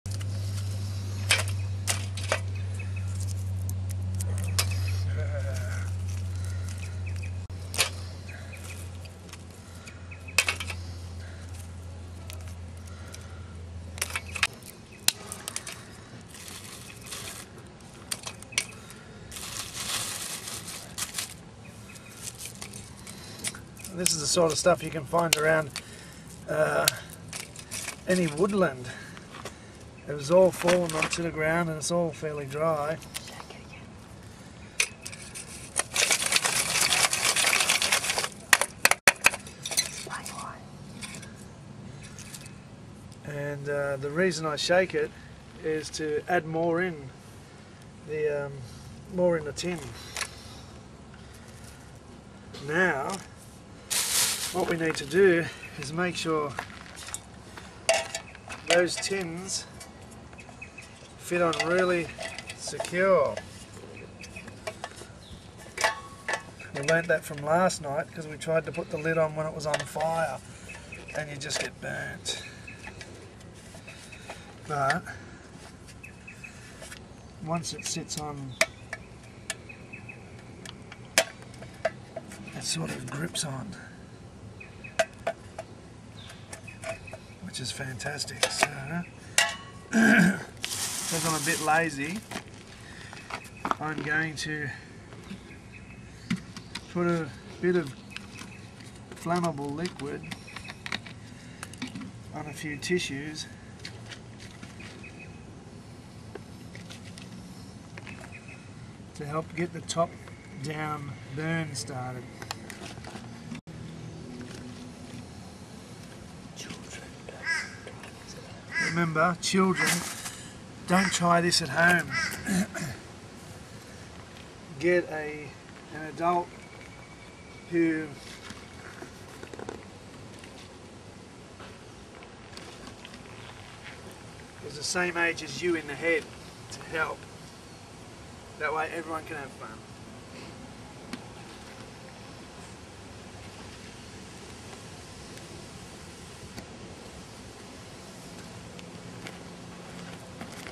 Biochar Camp Stove by biocharproject.org p2
At avatars abode in June 2010 I attempted to make a biochar camp stove just from verbal instructions given to me from Dr Paul Taylor just before he went back to the USA. This is part 2 of my attempts and hopefully it helps you discover biocharproject.org